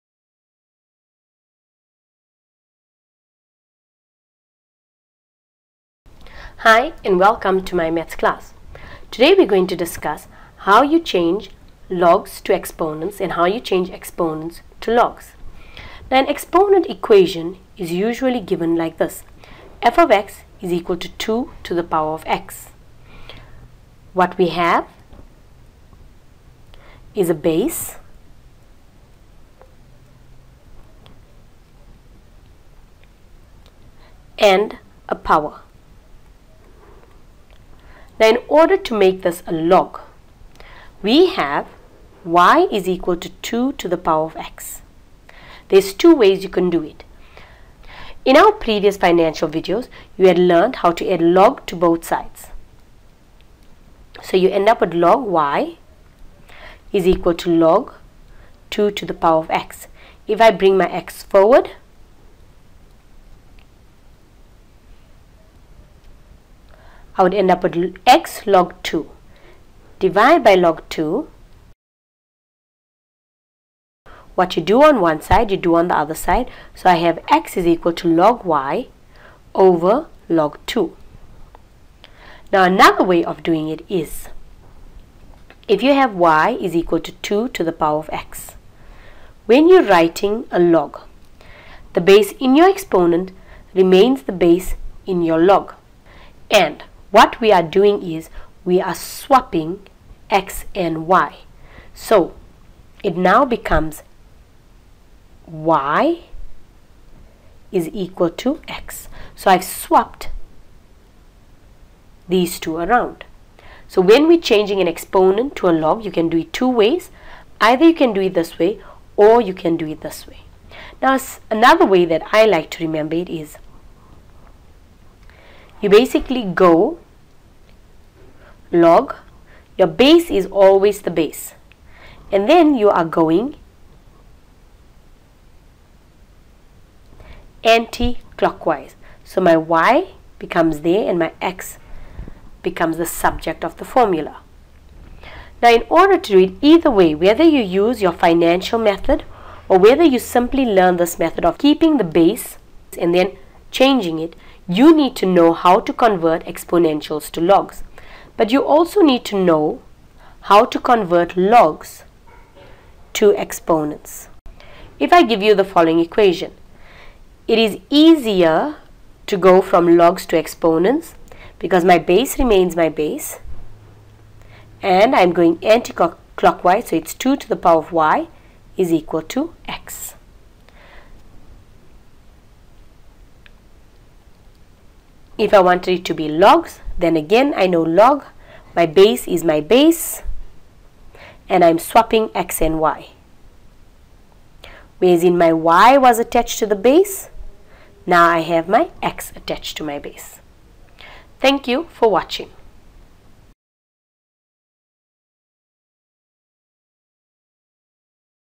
Grade 12: Functions: How to change logs to exponents and visa versa
Hey Guys